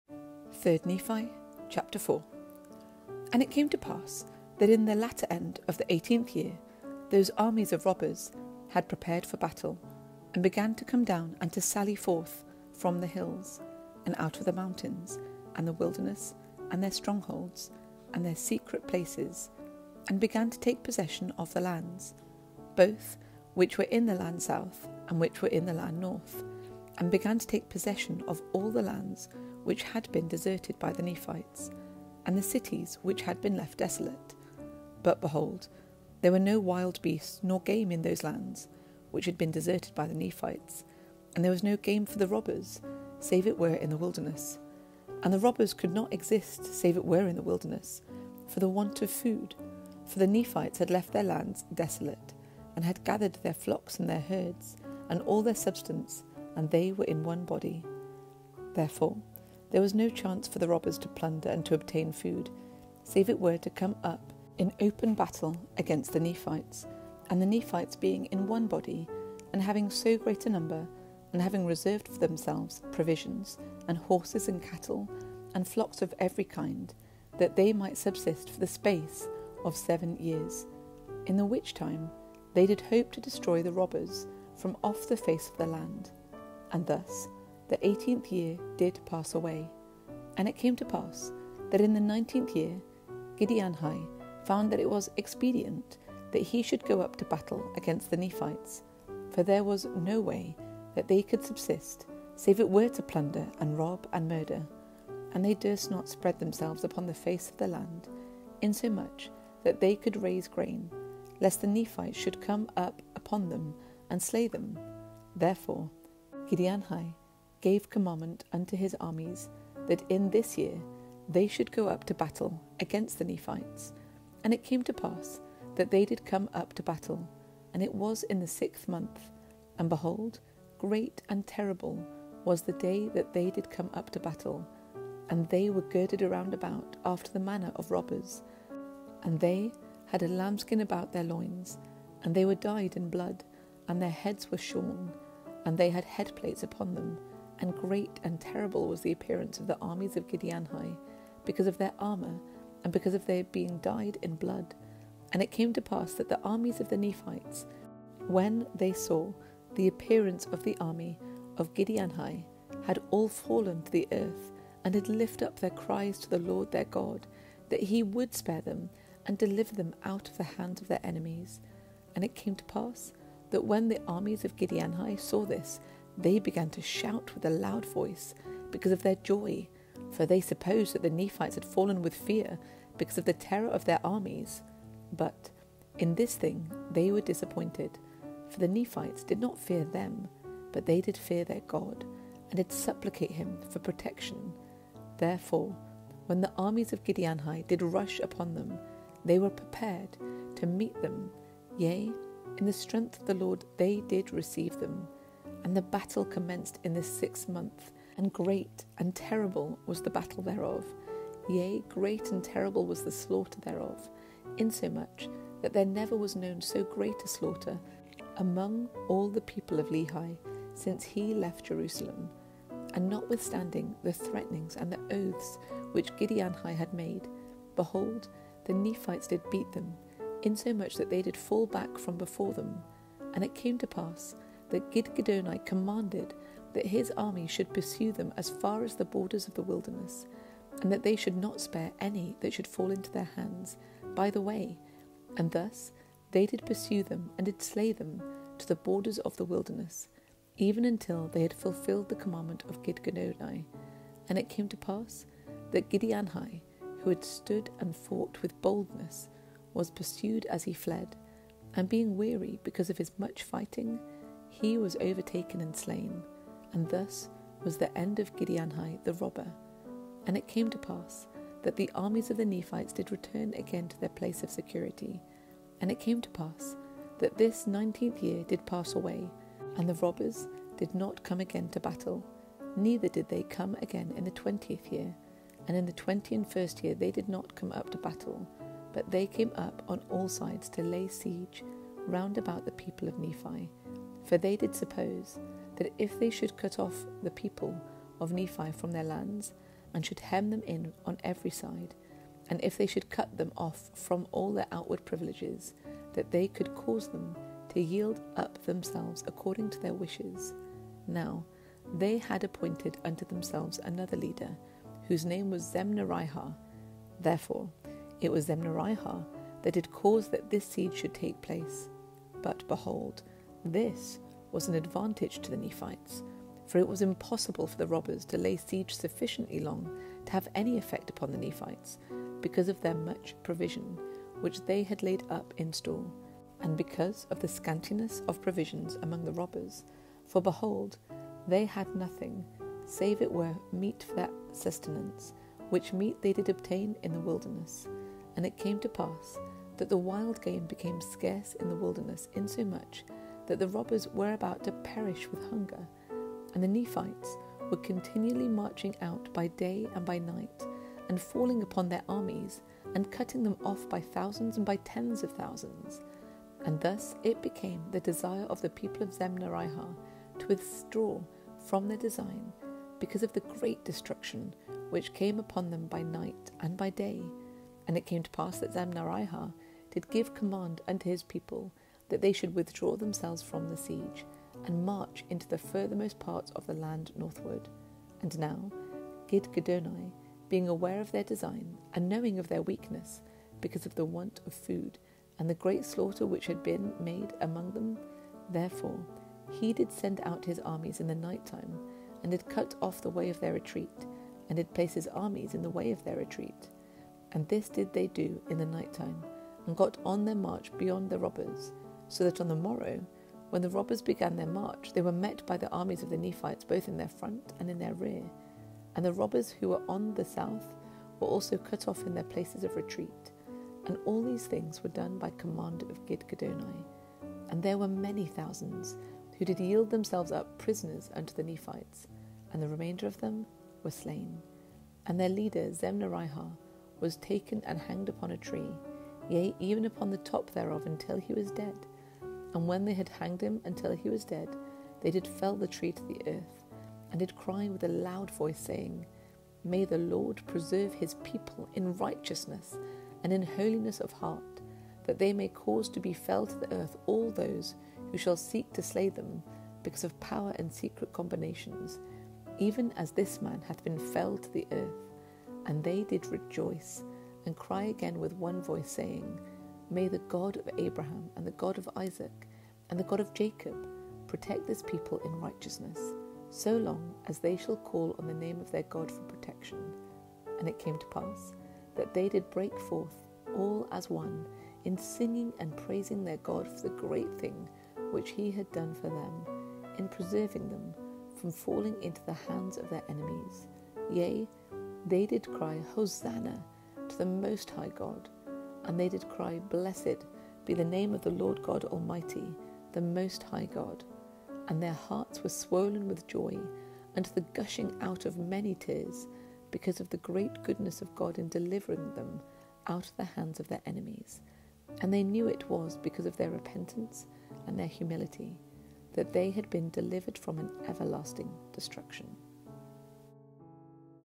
3 Nephi 4
3 Nephi 4 from The Book of Mormon, as read by The Church of Jesus Christ of Latter Day Saints.

British female vocals